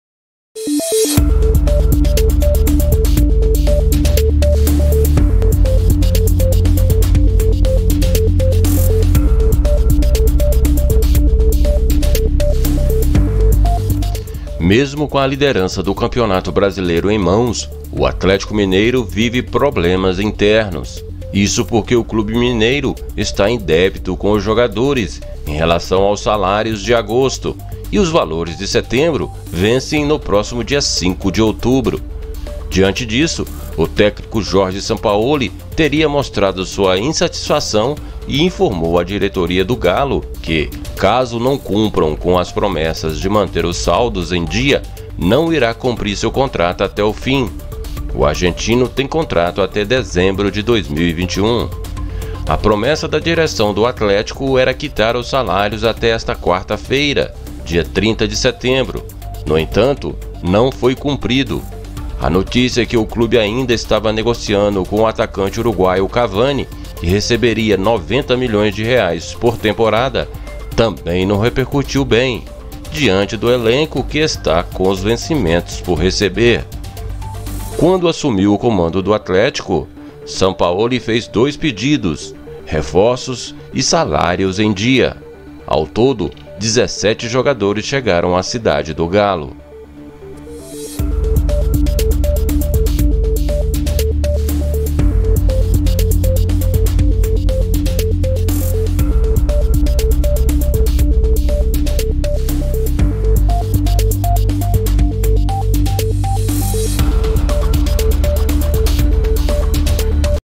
√ Atlético I Sampaoli ameaça romper contrato I 01.10.2020
Sampaoli se irrita com salários atrasados e ameaça romper contrato com o Atlético-MG. Medida do treinador argentino se dá por conta dos salários atrasados no clube mineiro.

Mesmo com a liderança do Campeonato Brasileiro em mãos, o Atlético-MG vive problemas internos. Isso porque o clube mineiro está em débito com os jogadores em relação aos salários de agosto e os valores de setembro vencem no próximo dia 5 de outubro.

Diante disso, de acordo com a Rádio Itatiaia, o técnico Jorge Sampaoli teria mostrado sua insatisfação e informou à diretoria do Galo que, caso não cumpram com as promessas de manter os saldos em dia, não irá cumprir seu contrato até o fim. O argentino tem contrato até dezembro de 2021.

A promessa da direção do Atlético-MG era quitar os salários até esta quarta-feira (30 de setembro). No entanto, não foi cumprido. A notícia que o clube ainda estava negociando com o atacante uruguaio Edinson Cavani, que receberia R$ 90 milhões por temporada, também não repercutiu bem diante do elenco, que está com os vencimentos por receber.

Quando assumiu o comando do Atlético-MG, Sampaoli fez dois pedidos: reforços e salários em dia. Ao todo, 17 jogadores chegaram à Cidade do Galo.

Jorge Sampaoli,
Gols da Rodada,
Brasileirão 2020,
ameaça romper com o Atlético,
Sampaoli se irrita com salários atrasados,